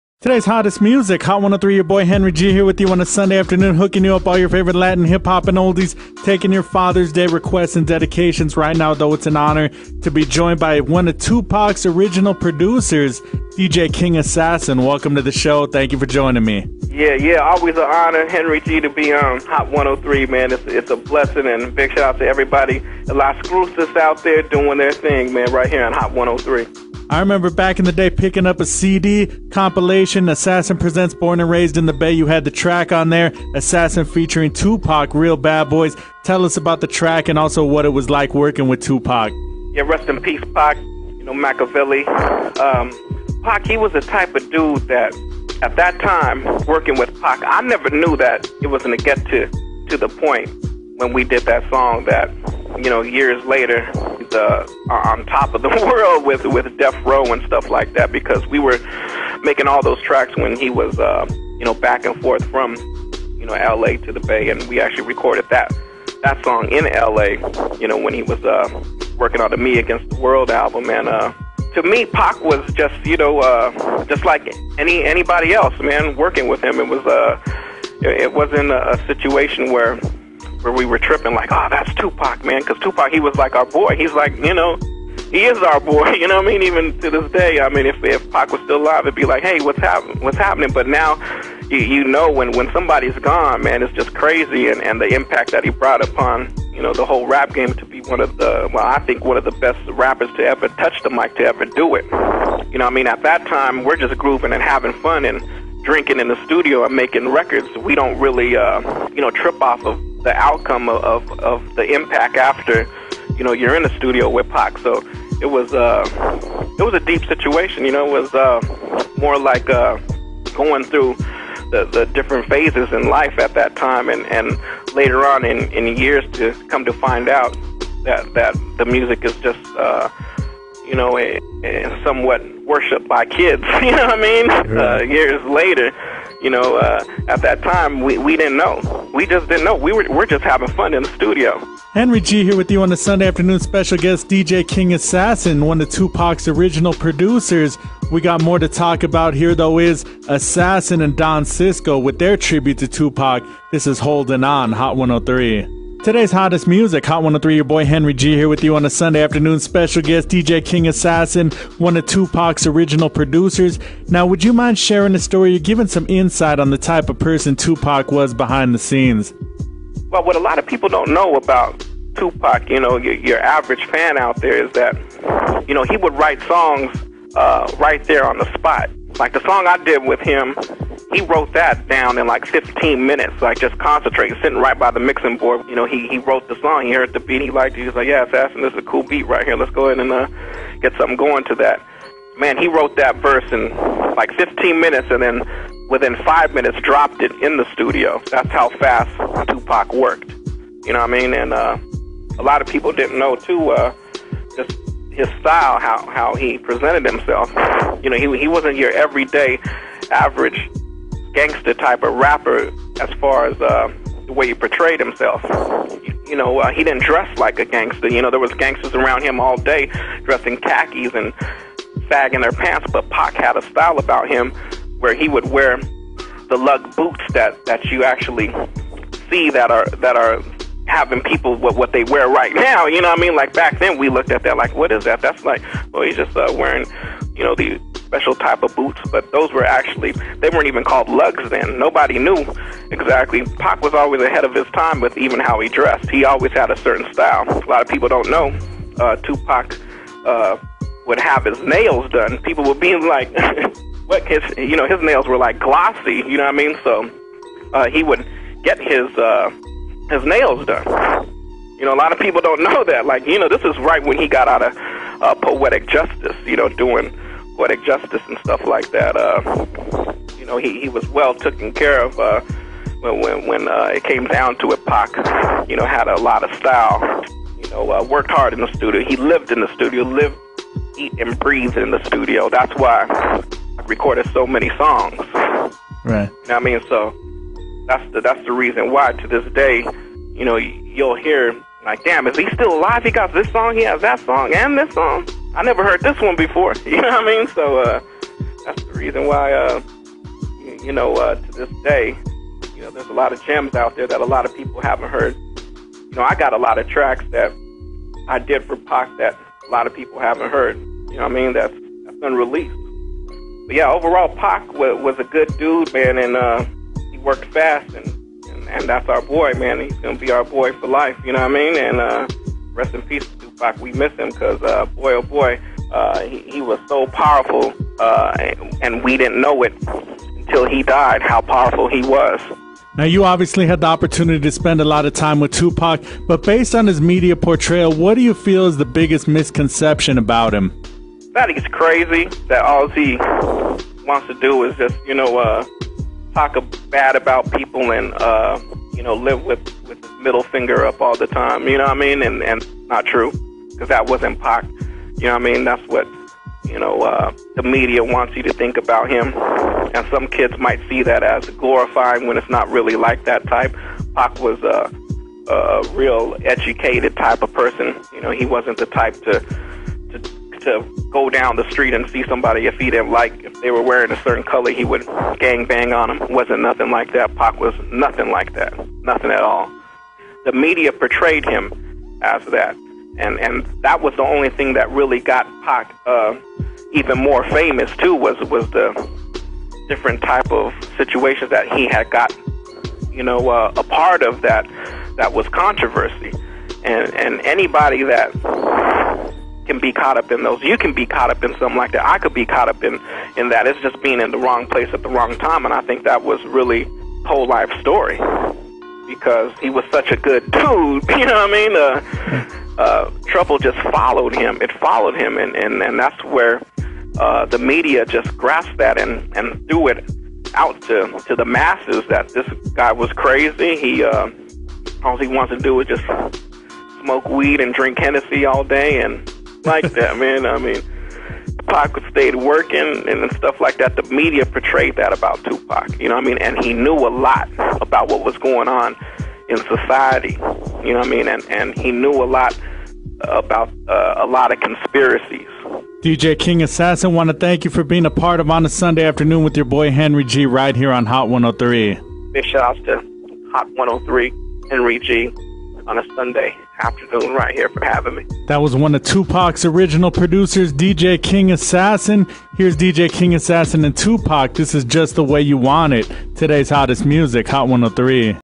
Exclusive Interview By Henry G 2Pac s Original OG Producer
DJ King Assassin exclusive interview with Henry G 2Pac s Original OG Producer